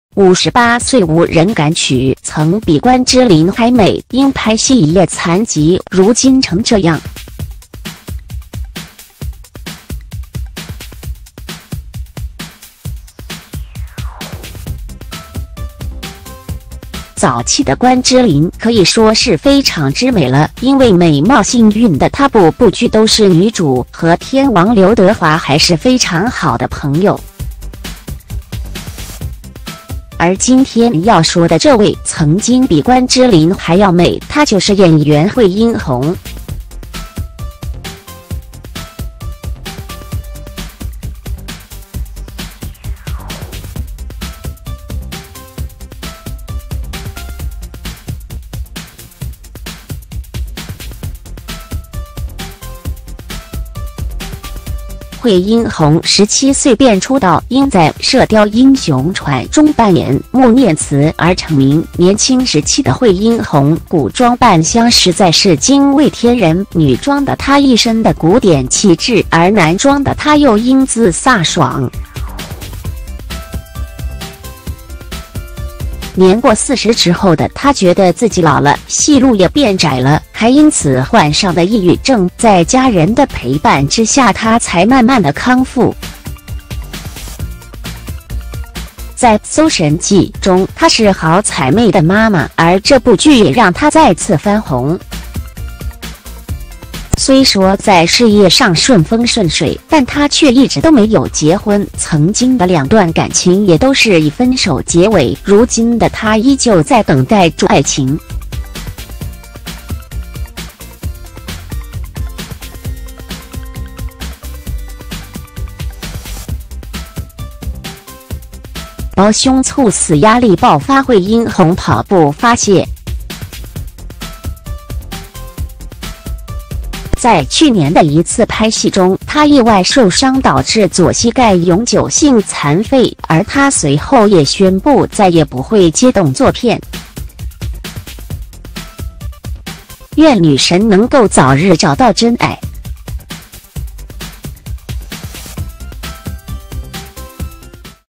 58歲無人敢娶，曾比關之琳還美，因拍戲一夜殘疾，如今成這樣！
58歲無人敢娶，曾比關之琳還美，因拍戲一夜殘疾，如今成這樣！